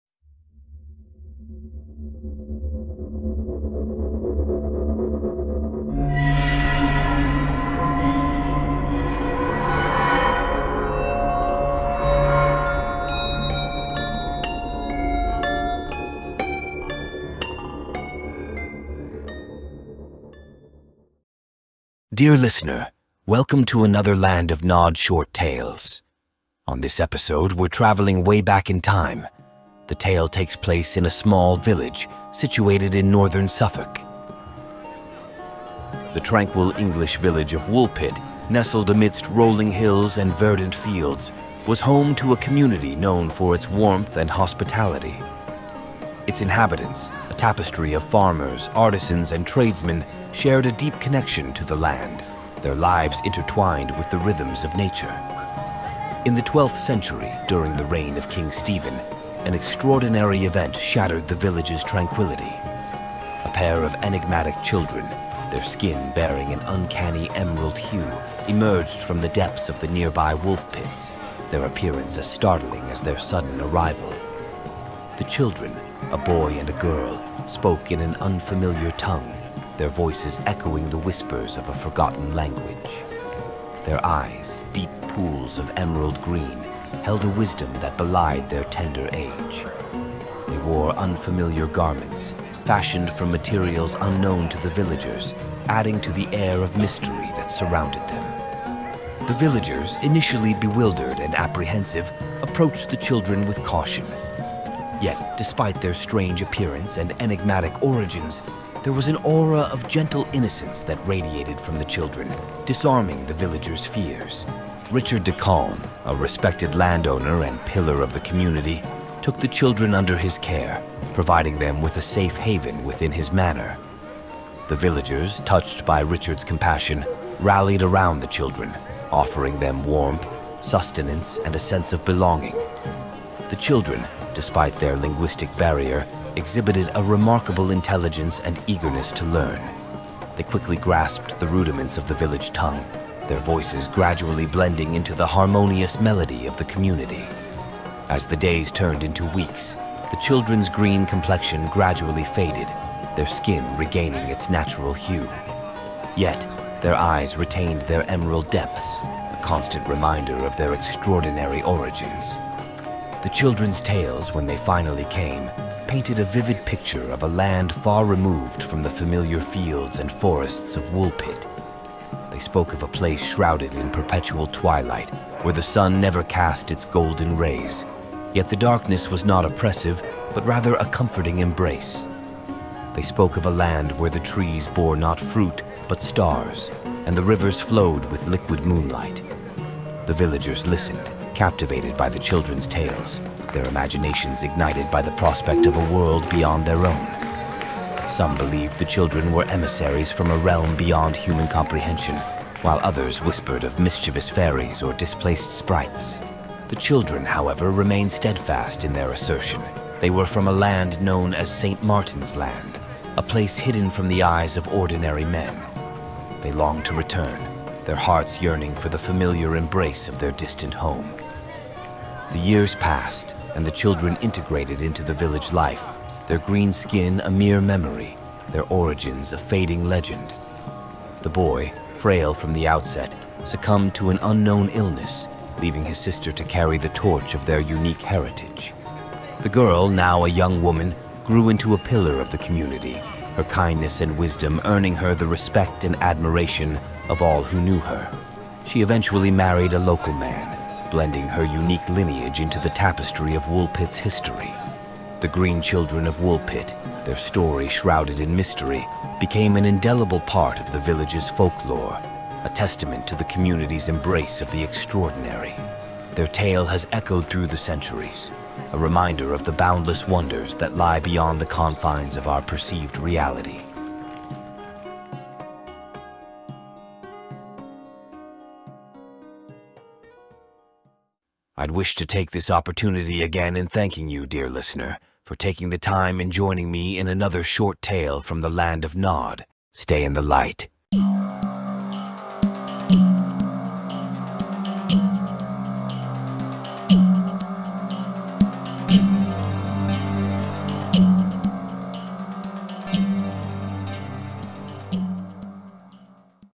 The Green Children Of Woolpit: A Mysterious Tale
Today, we embark on an exploration into one of history's most captivating enigmas—the curious account of the Green Children of Woolpit.

Stay in the light...